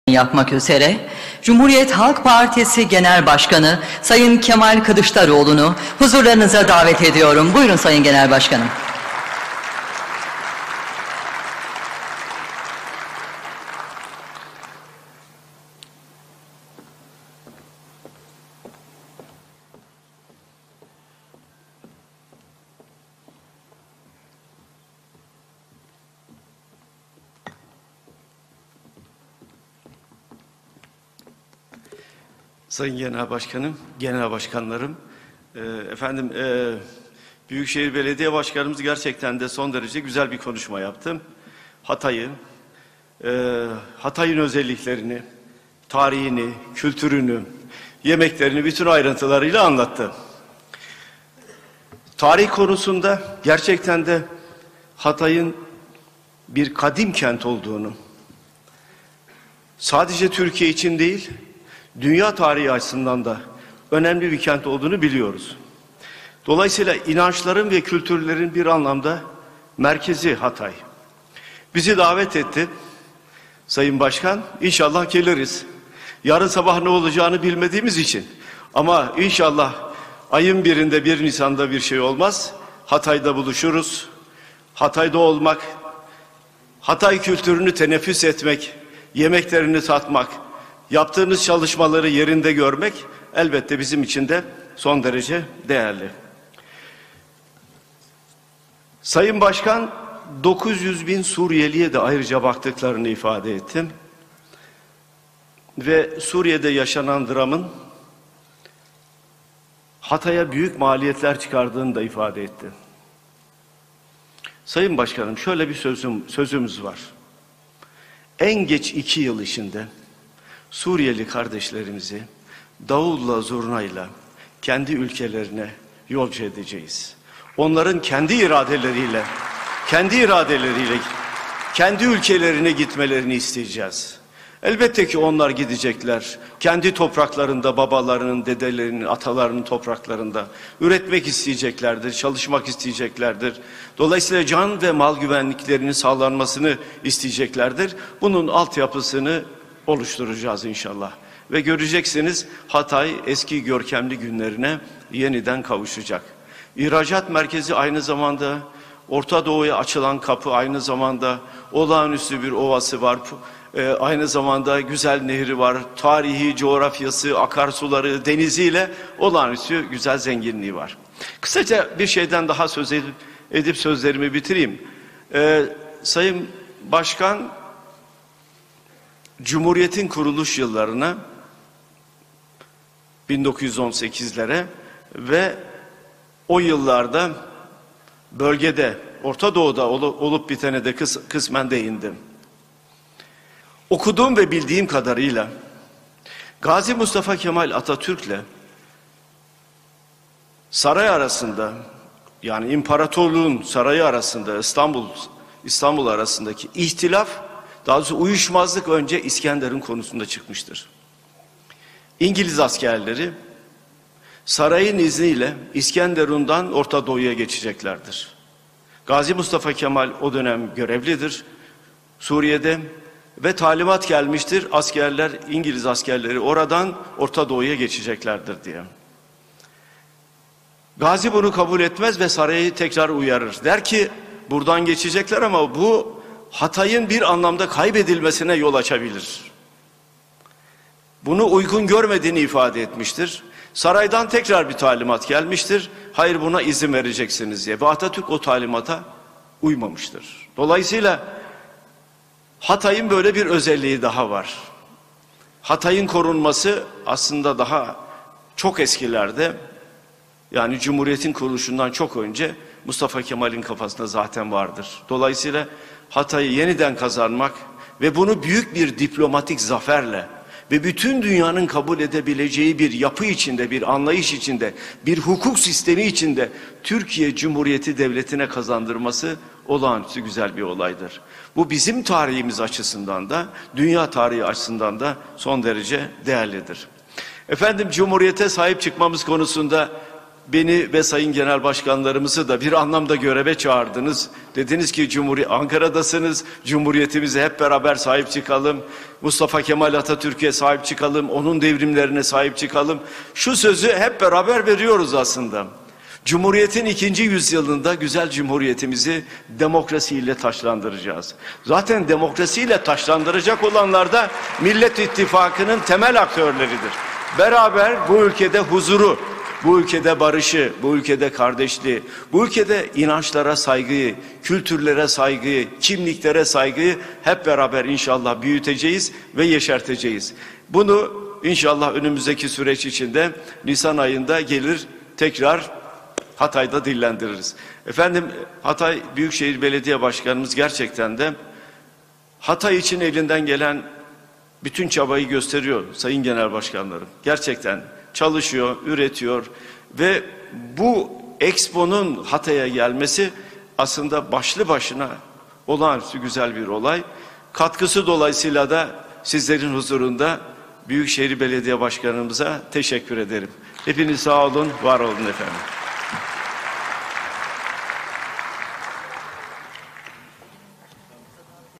KEMAL KILIÇDAROĞLU - HATAY EXPO LANSMANI 17/01/2022
Cumhuriyet Halk Partisi Genel Başkanı Kemal Kılıçdaroğlu, Hatay Expo Lansmanı'nda konuştu.